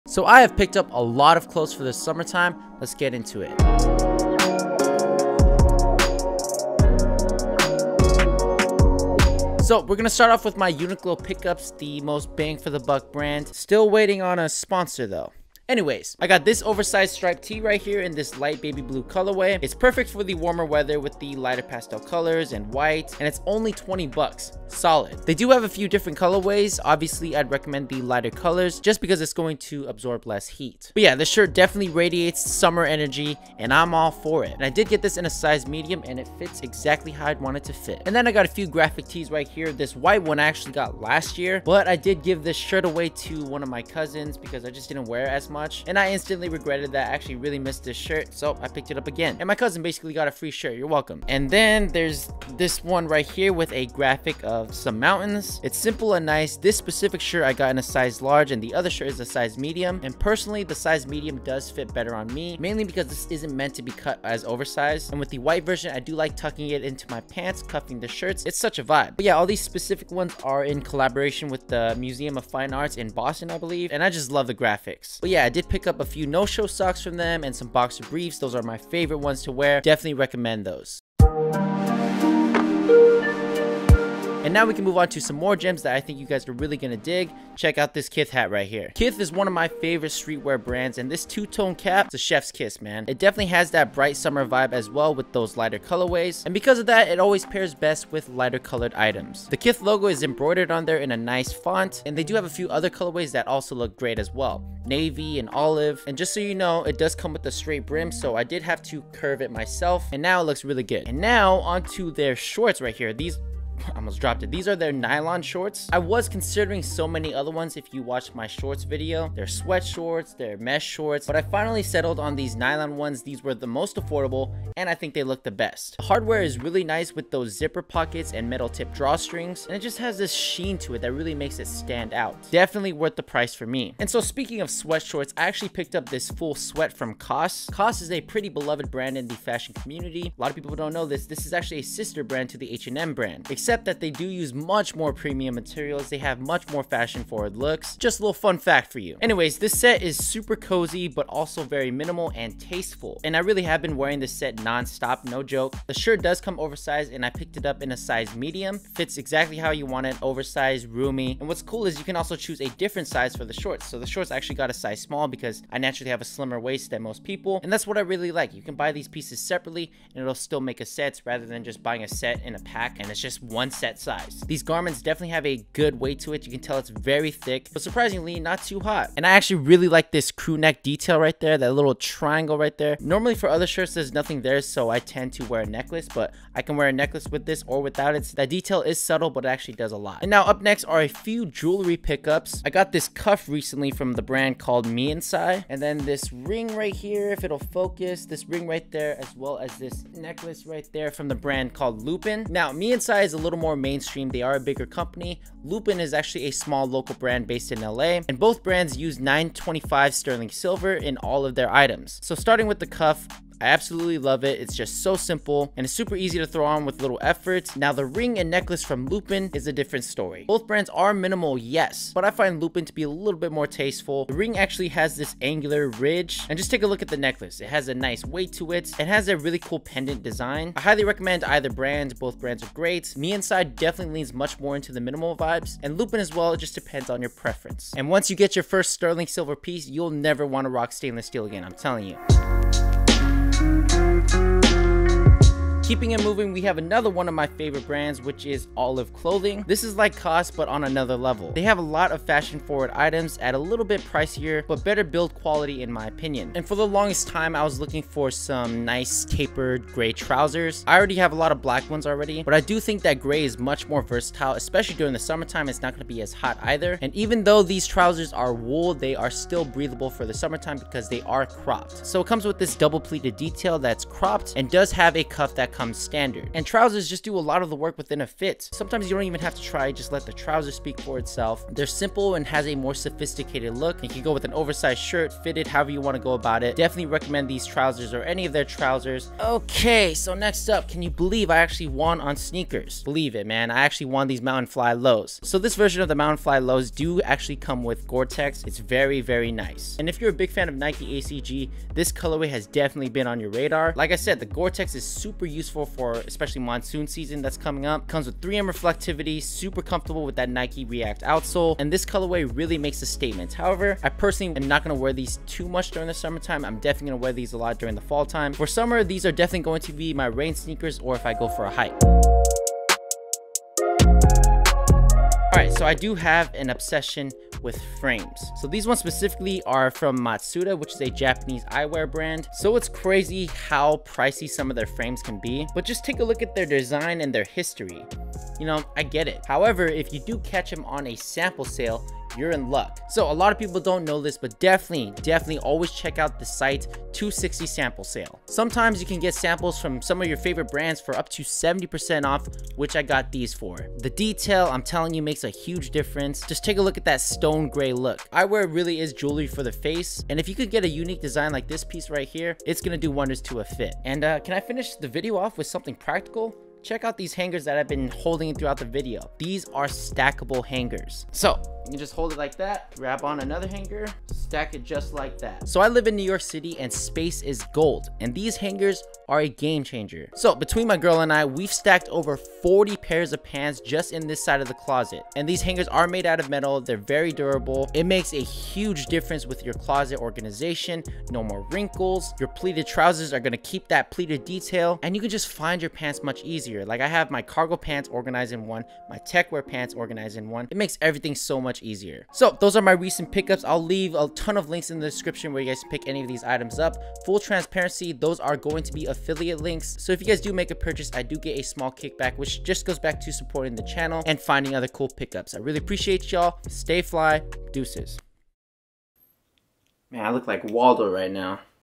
SUMMER PICKUPS (KITH, Uniqlo, COS, Summer Shorts & Jewelry)
Shop my pickups here:

Uniqlo Oversized Airism Tee
Uniqlo Striped Oversized Tee
Uniqlo Graphic Tee (white)
Uniqlo Graphic Tee
Uniqlo Airism Briefs
Uniqlo Low Cut Socks

Stackable Hangers

Other items worn throughout the video:

Nike Air Max 90
Oliver Cabell White Sneakers
Casio Gold Watch